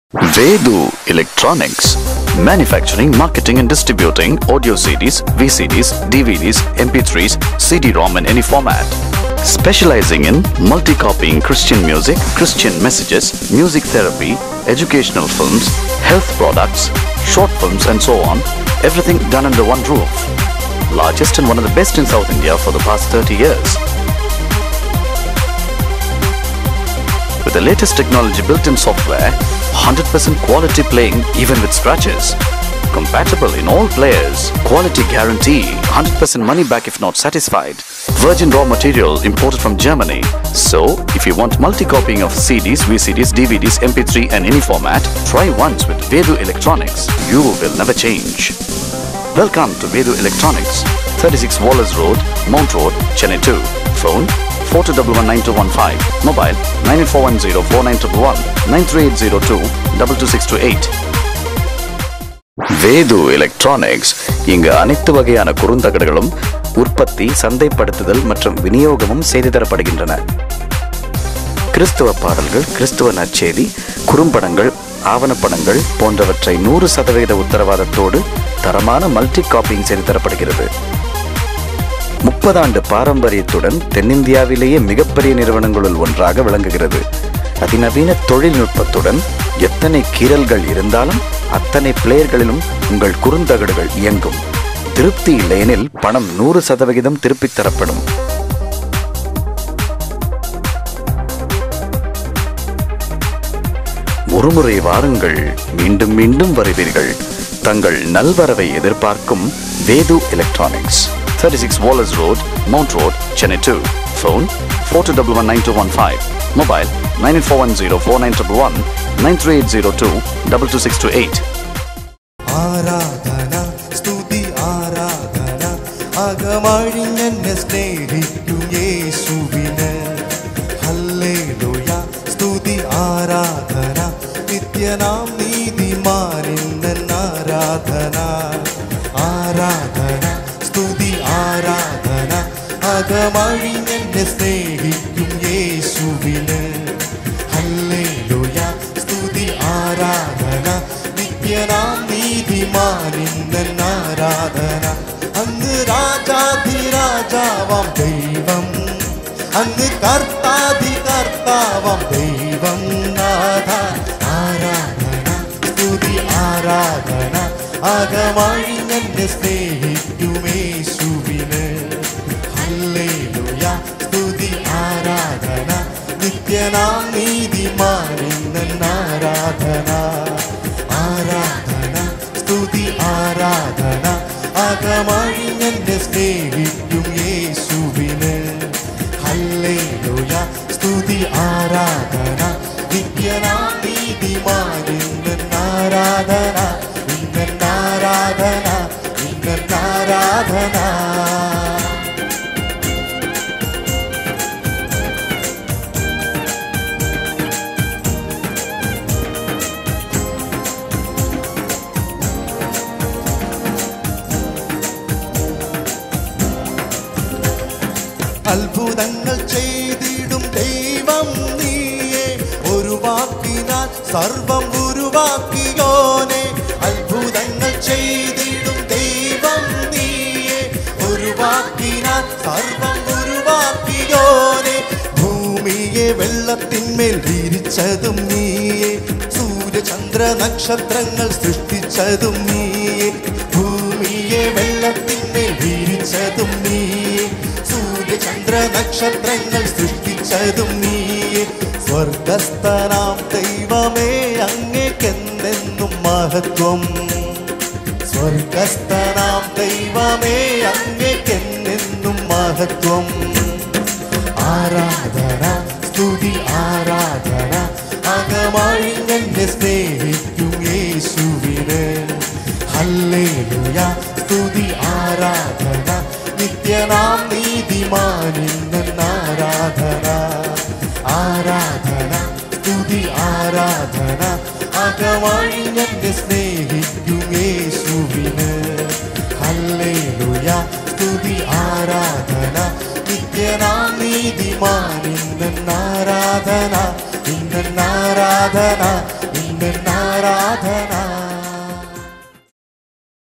End Card | Jehovah Nissi Year of Build Up | Rev. N Peter
This Show is about Jehovah Nissi Year of Build Up. A Message by Rev.N Peter
Hosts : Pr. Anil Babu,Bro. Aldrin,Bro. Madhu Lal,Bro. Austin,Bro. R S V
Produced by Vedu Electronics